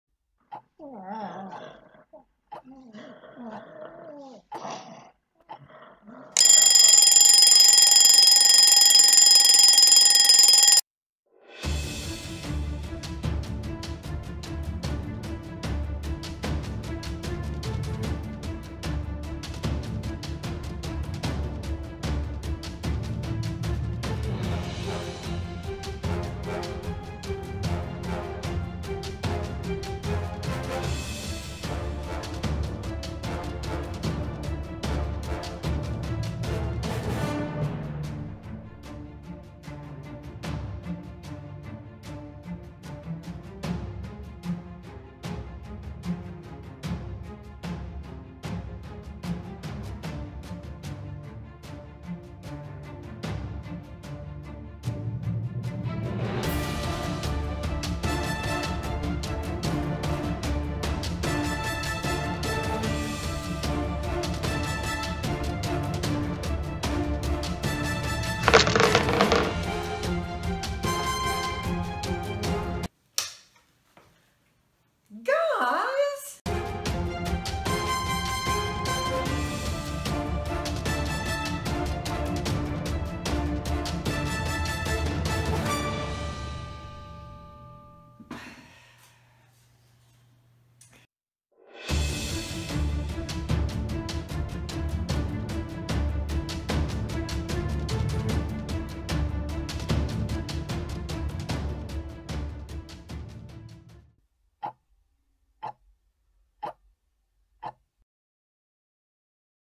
Gnomes breakfast
A stop motion video of our favorite guys getting breakfast.